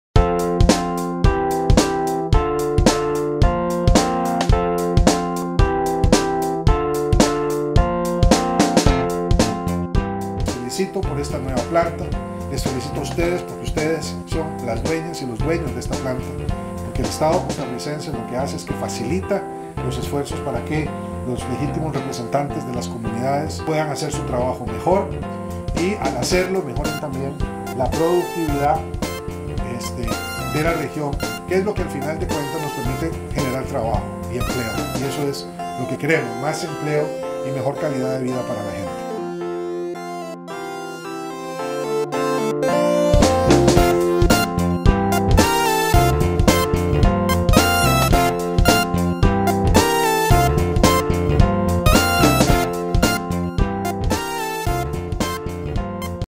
Nueva Planta Asfáltica de la Zona Sur de Costa Rica
La nueva planta asfáltica producirá el material para los municipios de Pérez Zeledón, Golfito, Corredores, Osa, Buenos Aires y Coto Brus.